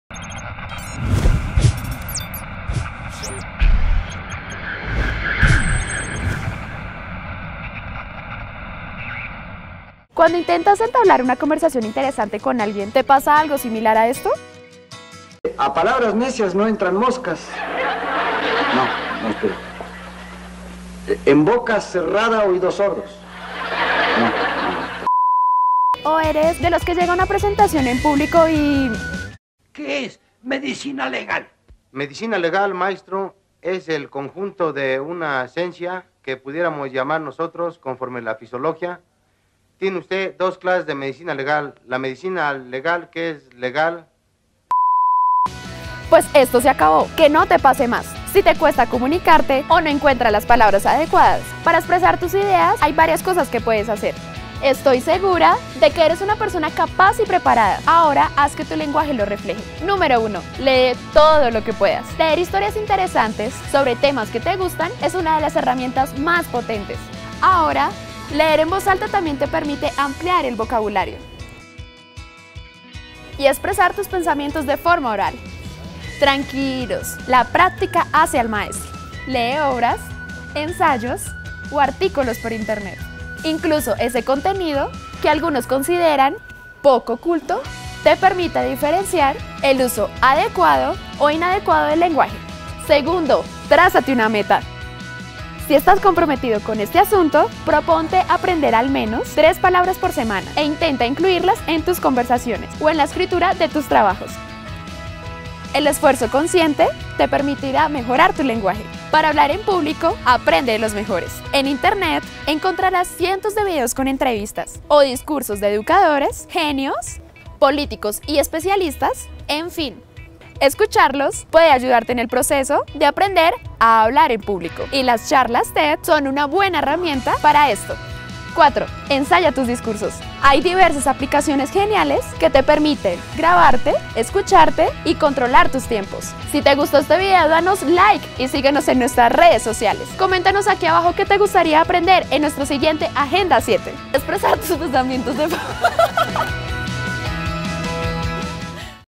¡Que nunca te quedes sin palabras! | Agenda 7TV
Ten presente estos tips que te ayudarán a ampliar tu vocabulario y expresar muy bien tus ideas.